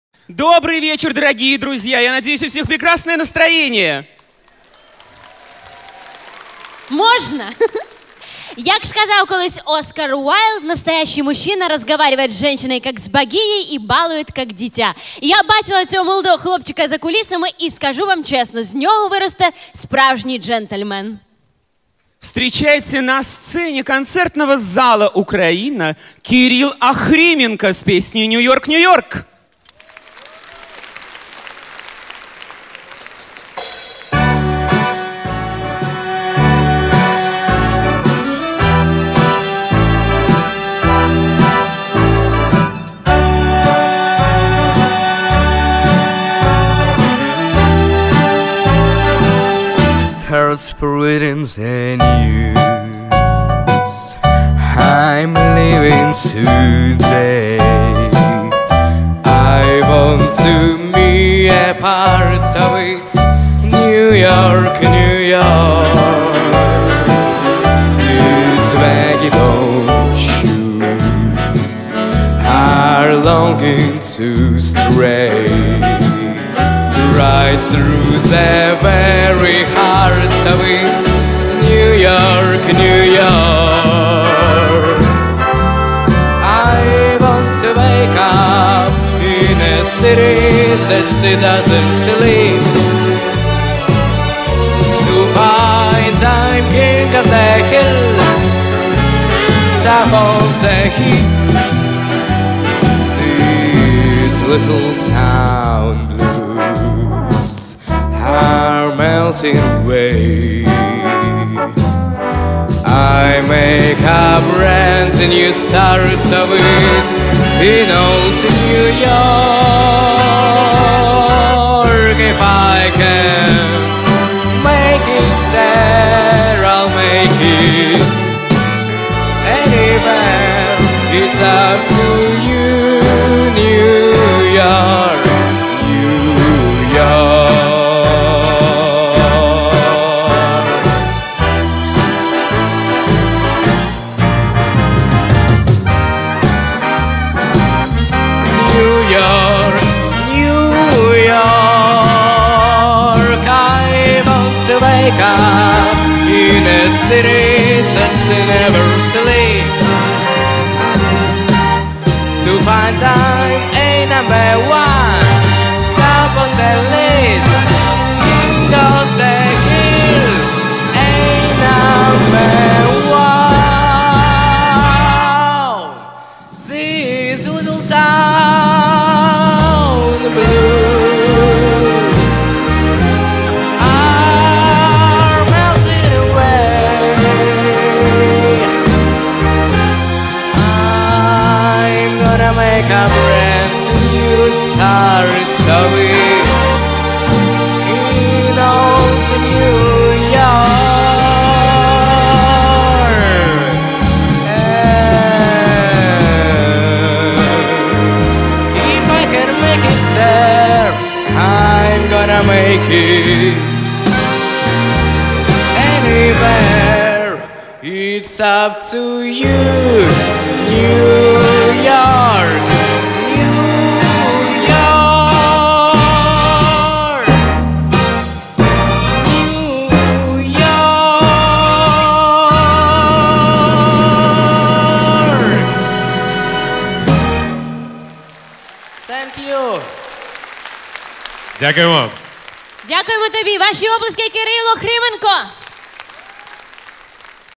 Кирилл Охрименко (юбилей ФРИСТАЙЛ, live) дв. Украина
Кирилл Охрименко, NEW YORK, дв.Украина, юбилейный концерт ФРИСТАЙЛ.
Ведущие Ольга Цыбульская, Mонро,Тимур Мирошниченко.
В концерте: группа ФРИСТАЙЛ, Нина КИРСО, Сергей Зверев, В.Бондарчук, В.Козловский, П.Черный, Н.Бучинская,А.Кудлай, ММдэнс, М.Грицкан, "ВЕРАСЫ", Денис Супруненко,